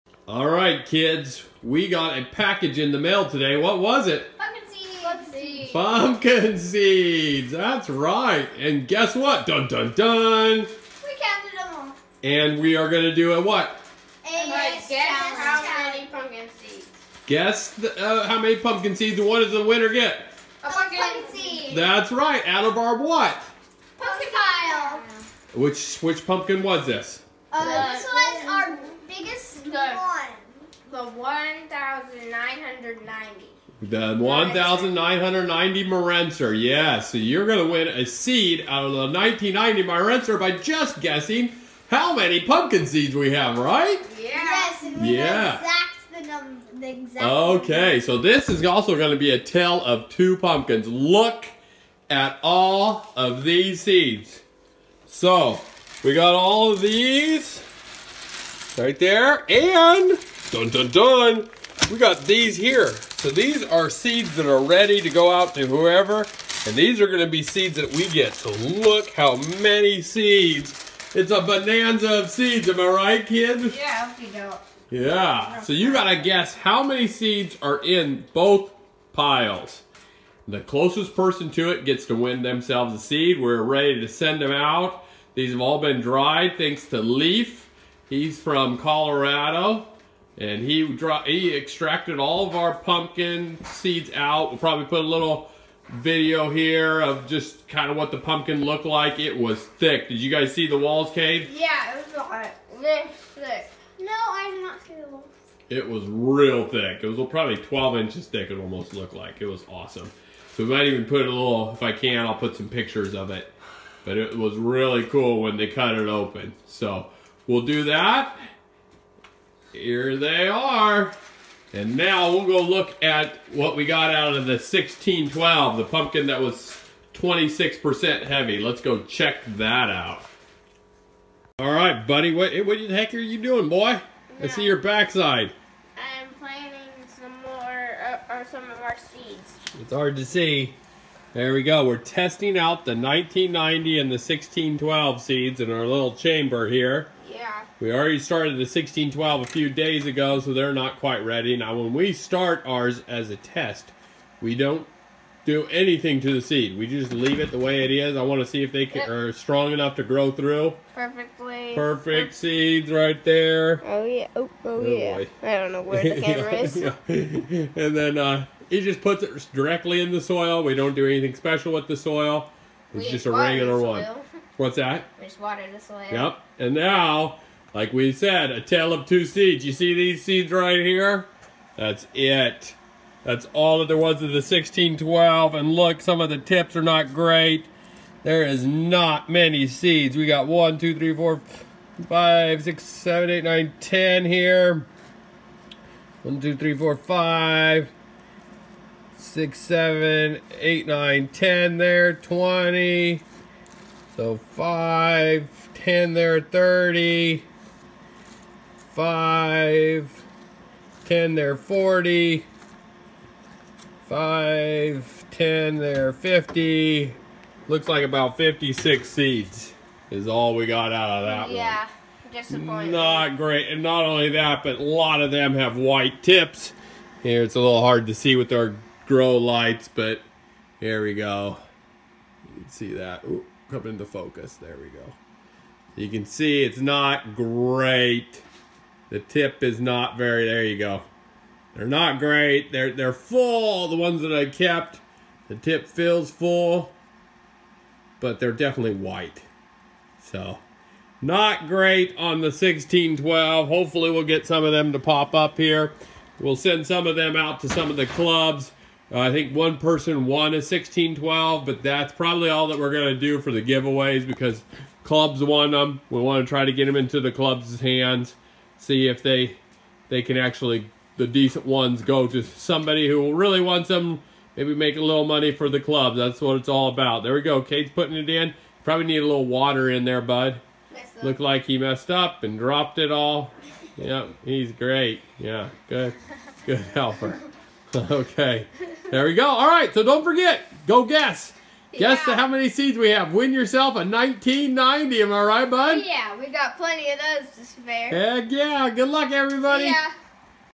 How Many Pumpkin Seeds are in the Pile. Closest guess wins a seed!
If you would like to purchase some seeds you can go

We finally cut open our 1612 marintzer pumpkin and our 1990 marintzer pumpkin. Now it is your time to guess how many seeds we received out of our 1990 Marintzer whoever is the closest wins a seed if you are exactly correct we'll give you one of each of our pumpkin seeds!